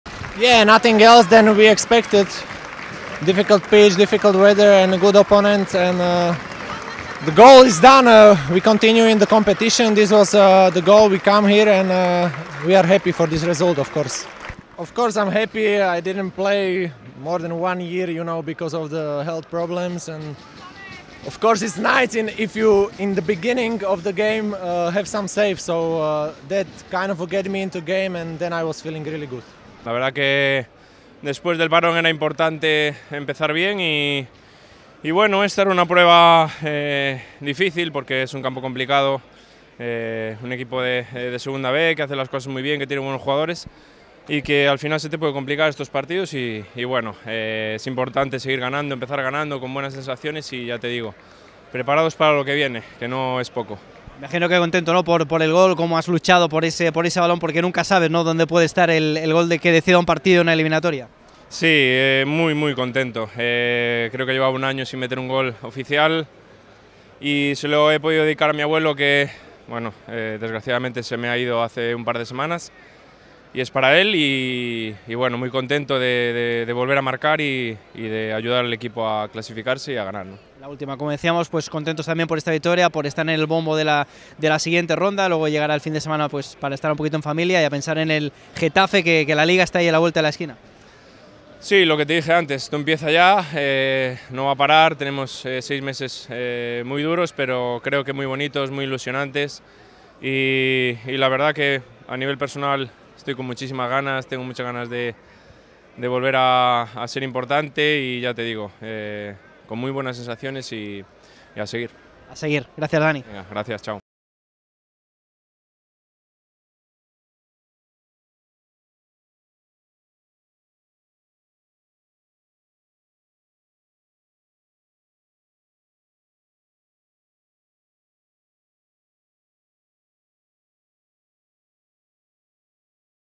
🎙️ 다니 로드리게스와 그리프의 국왕컵 경기 승리 소감 | RCD Mallorca
마요르카는 다니 로드리게스의 득점과 그리프의 연이은 선방에 힘입어 이번 경기에 승리했습니다.